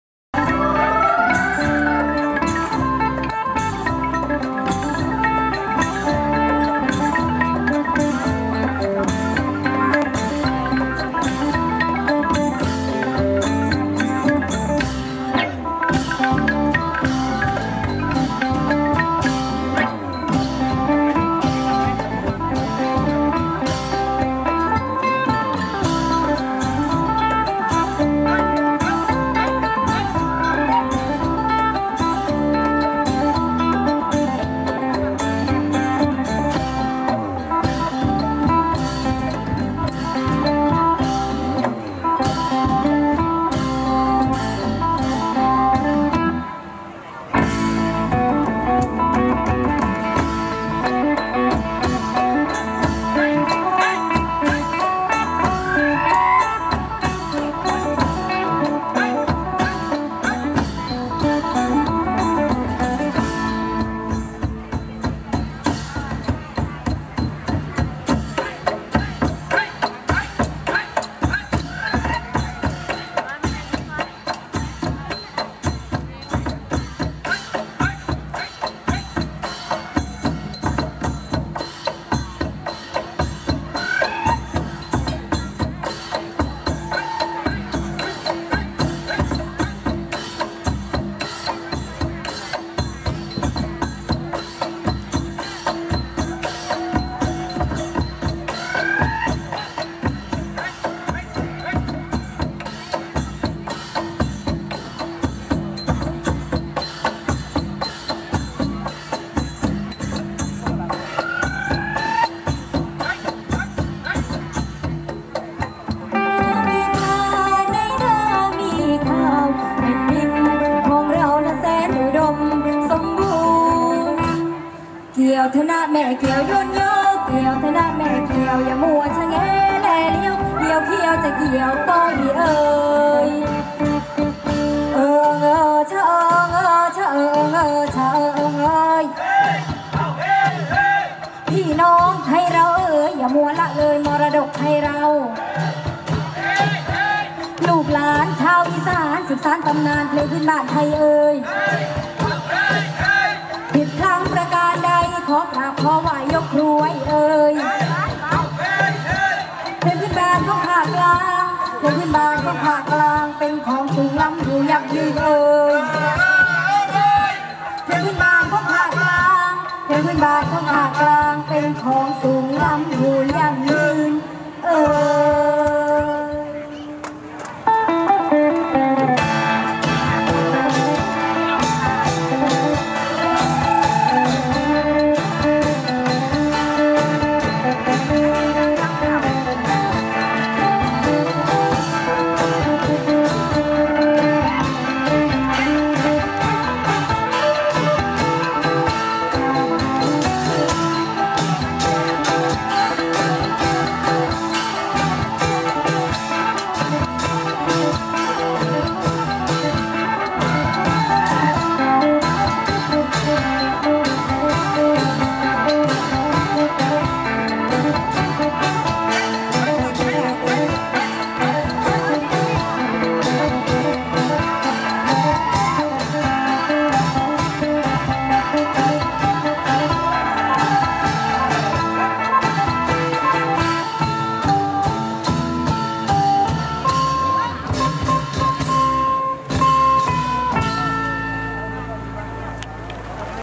Northeastern Folk Festival : Paka Lamduan Troupe, Sisaket Province
[Live Video] Northeastern Folk Festival : Paka Lamduan Troupe, Sisaket Province [DCP] การแสดงพื้นบ้านภาคตะวันออกเฉียงเหนือ โดยวงผกาลำดวน มหาวิทยาลัยราชภัฏศรีสะเกษ ภายในงาน ใต้ร่มพระบารมี๒๓๖ปีกรุงรัตนโกสินทร์ ณ ลานคนเมือง ศาลาว่าการกรุงเทพมหานคร โดยวันนี้จัดเป็นวันสุดท้าย